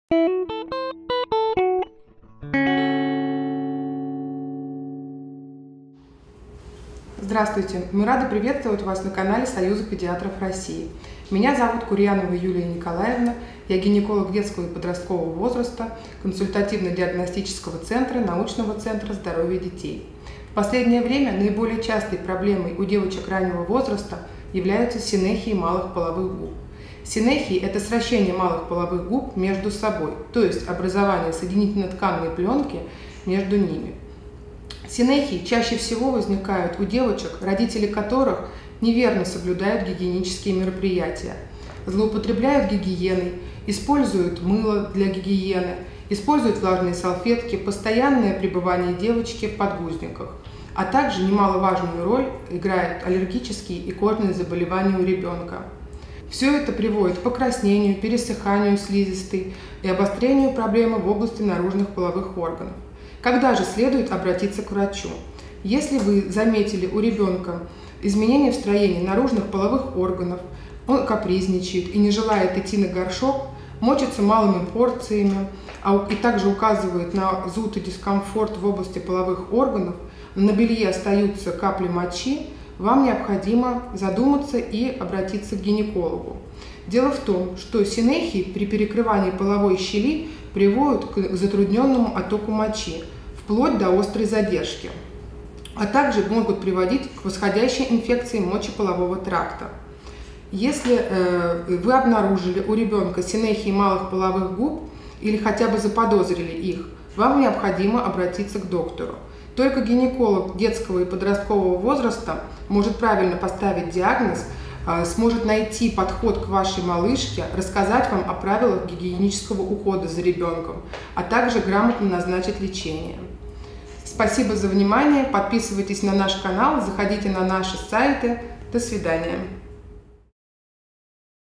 Синехии (сращение) малых половых губ у девочек. Советы родителям - Союз педиатров России.
Союз педиатров России.

Синехии (сращение) малых половых губ у девочек - что это, что нужно знать? Советы родителям.

Рассказывает Ю.Н. Курьянова, детский гинеколог,